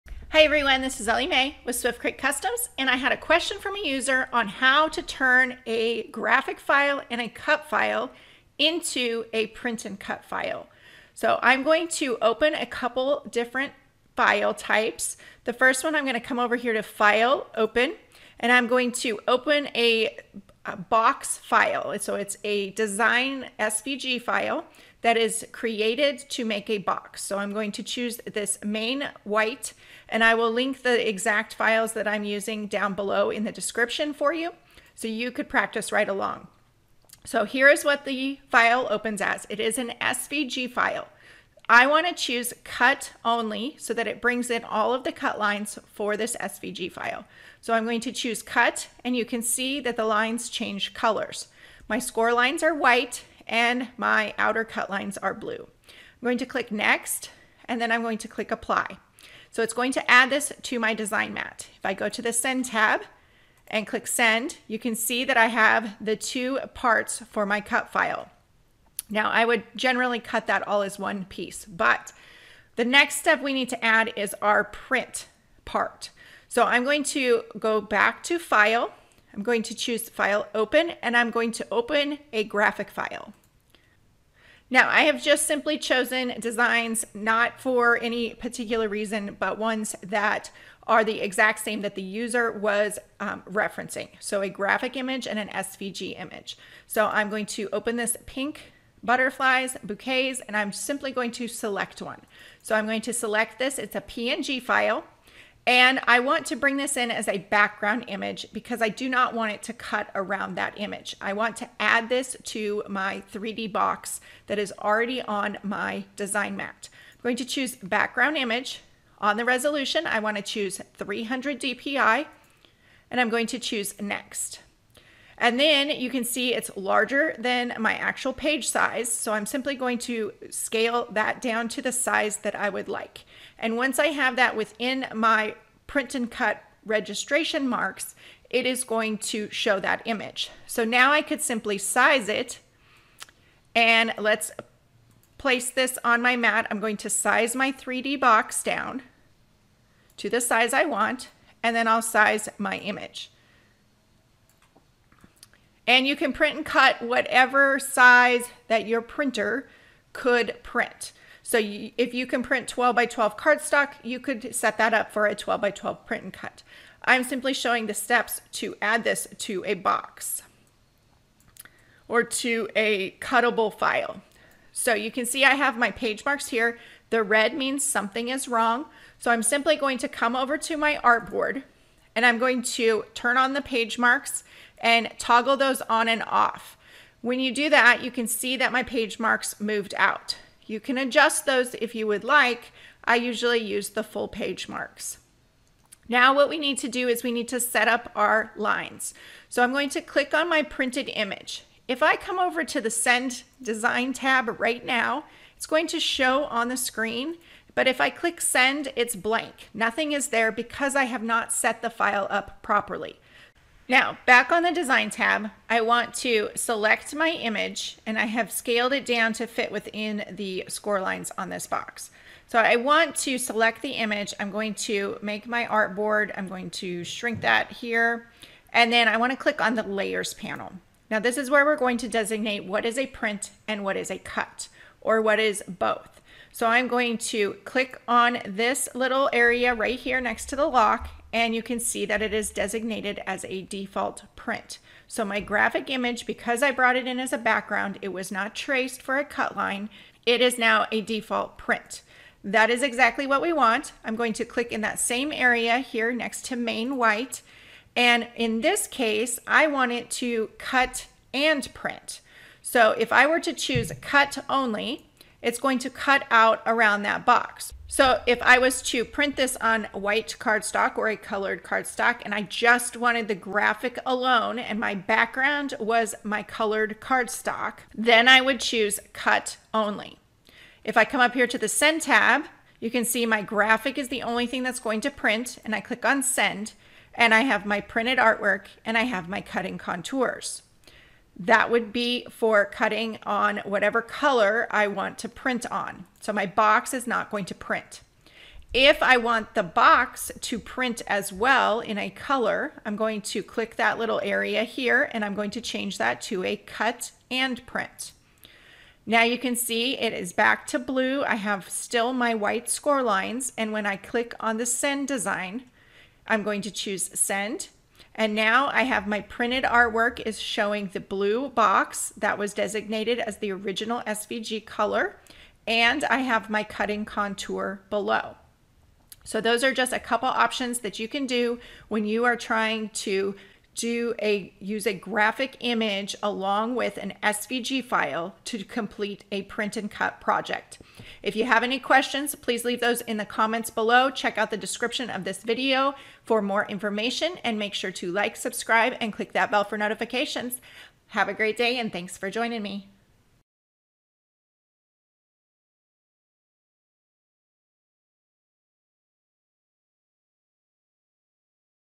Siser Leonardo Design Studio - Print and Cut with graphics and a SVG file
In this quick tutorial, I'm sharing how to open a SVG cut file and then open a png graphic image and set it up as a print and cut in the Siser Leonardo Design Studio software.